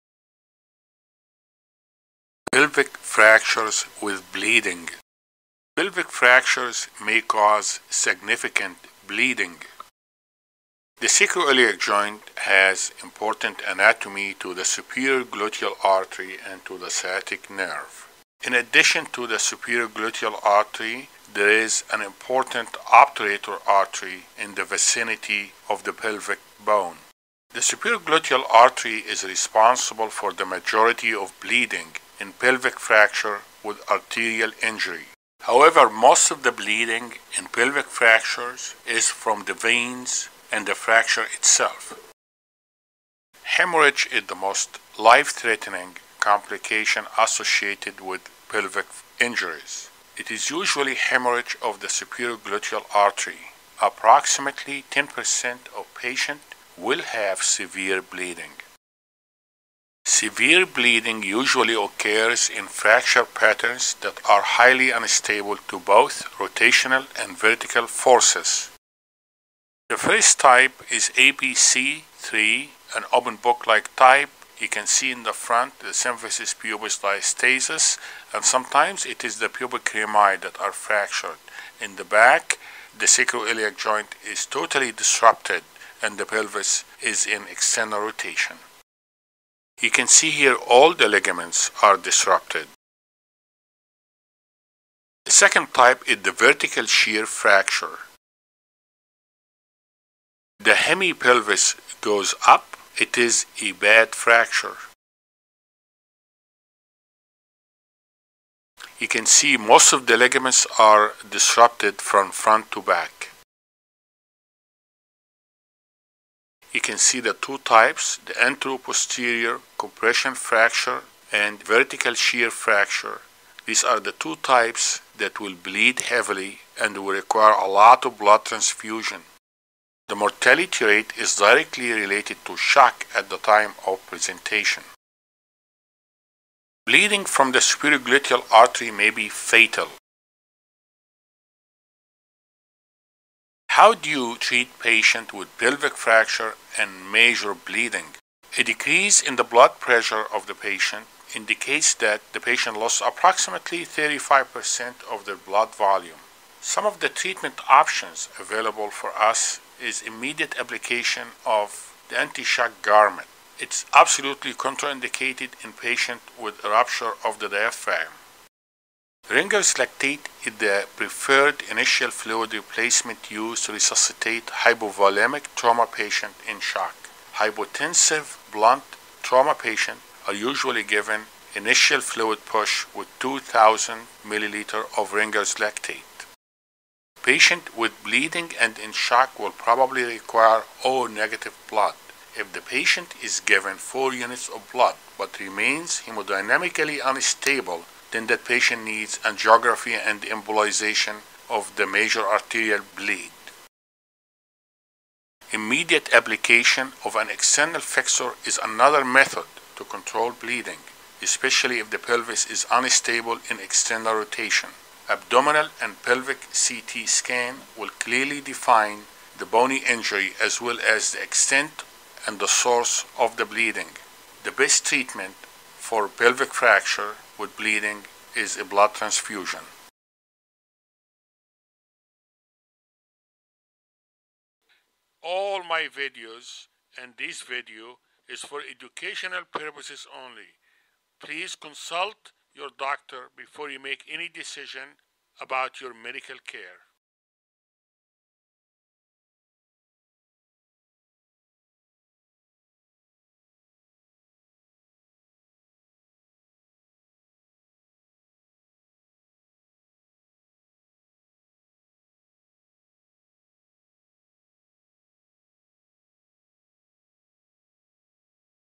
Pelvic Fractures With Bleeding - Everything You Need To Know - Dr. Nabil Ebraheim
Dr. Ebraheim’s educational animated video describes fracture injury conditions of the pelvis associated with bleeding and hemorrhage.
Pelvic fractures may cause significant bleeding. The superior gluteal artery is responsible for the majority of bleeding in pelvic fractures with arterial injury. Most of the bleeding in pelvic fractures is from the veins and the fracture itself.
Hemorrhage is the most life-threatening complication associated with pelvic injuries. Approximately 10% of patients will have severe bleeding.
Sever bleeding usually occurs in fracture patterns that are highly unstable to both rotational and vertical forces.
1-APC III (open book like type). Symphysis pubic diastasis. Complete disruption of anterior SI, sacrotuberous and sacrospinous ligaments; disrupted posterior SI ligaments.
2-Vertical shear fracture: bad fracture. Complete disruption of the anterior and posterior ligaments; cephaloposterior displacement.
Anteroposterior compression or vertical shear injuries are consistently associated with a high risk of mortality from bleeding.
Mortality rate is directly related to shock at the time of presentation.
Bleeding of the superior gluteal artery could be fatal.
How do you treat patients with pelvic fracture and massive bleeding?
•Patient will lose approximately 35% of their blood volume with acute hemorrhage before a sustained decrease in systolic blood pressure occurs.
•Immediate application of a pneumatic antishock garment. Absolutely contraindicated in patient with a rupture of the diaphragm.
•Ringer’s lactate is the preferred initial fluid replacement used to resuscitate hypovolemic trauma patients in shock. Hypotensive blunt trauma patient is given initial fluid push with 2,000 ml of Ringer's lactate.
•Patient with bleeding and in shock will probably require O negative blood.
•If the patient is given 4 units of blood but remains hemodynamically unstable, then angiography and embolization are needed.
•Immediate application of an external fixator is another method to control bleeding, especially if the pelvis is unstable in external rotation.
•Abdominal and pelvic CT scan will clearly define the bony injury as well as the extent and source of bleeding.
•The best treatment for pelvic fractures with bleeding is a blood transfusion with correction of hypothermia and coagulopathy.